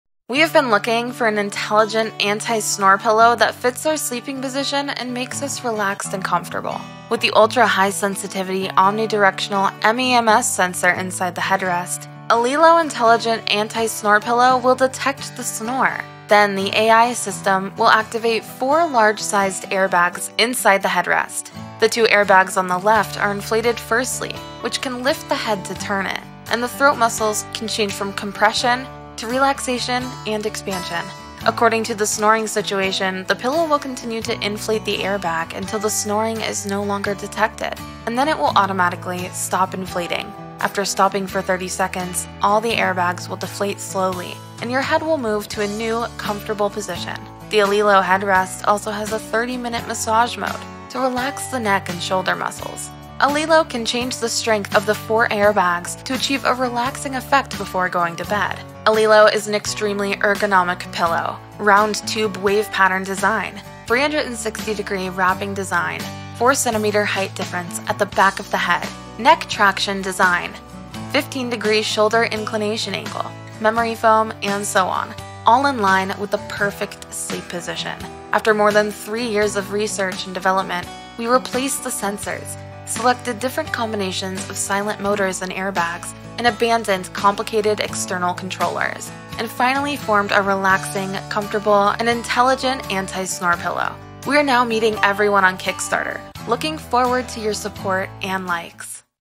Now on Kickstarter: Alisleep: A Pillow That Can Stop Snoring And Massage
AliSleep: A pillow that can stop snoring and massage
Anti-snoring / Massage / AI-powered / 28db low noise / Manual Control / All night standby / Comfortable pillow